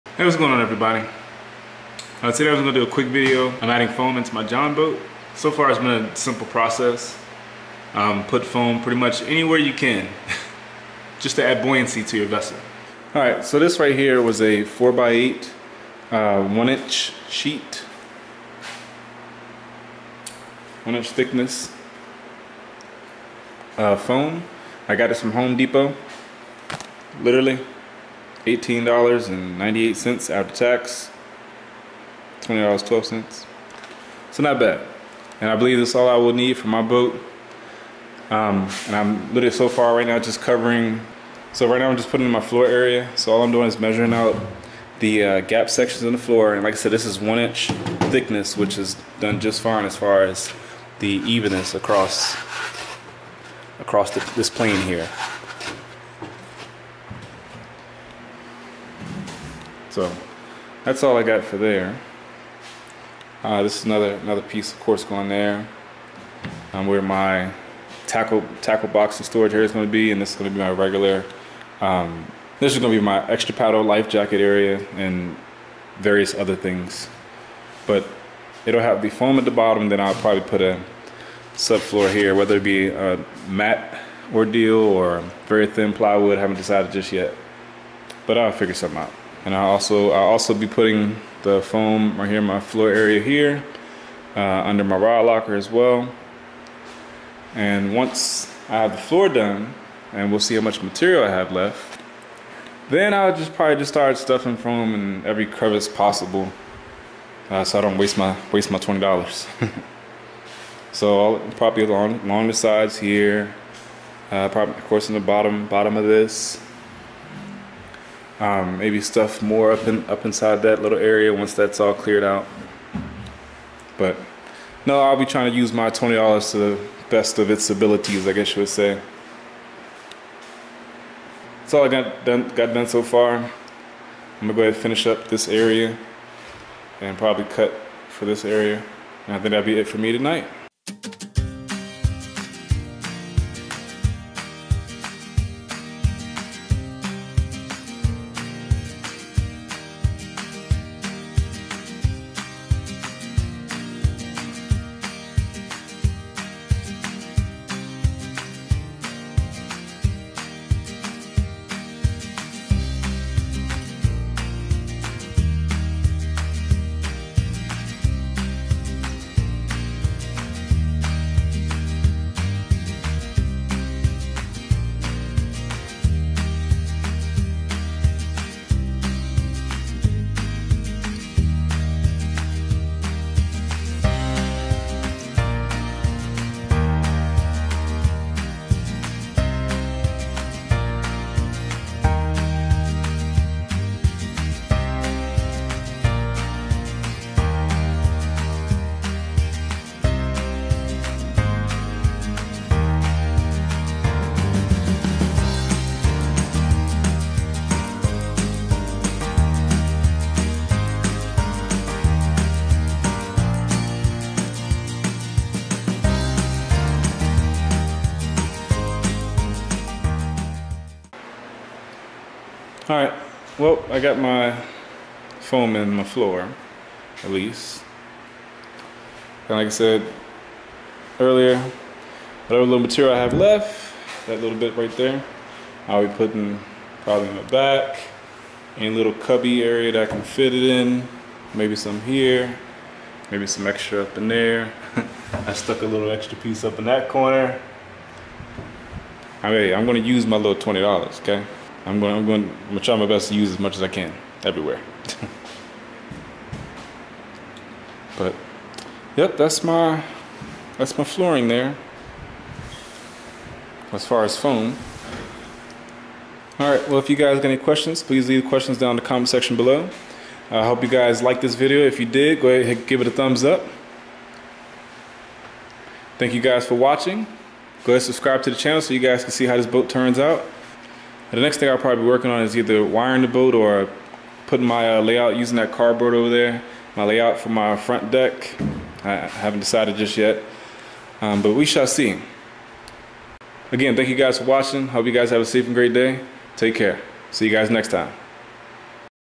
Adding Foam to my Jon Boat to Bass Boat Conversion
In this video I will be showing you how I added foam to my Jon Boat to Bass Boat Conversion.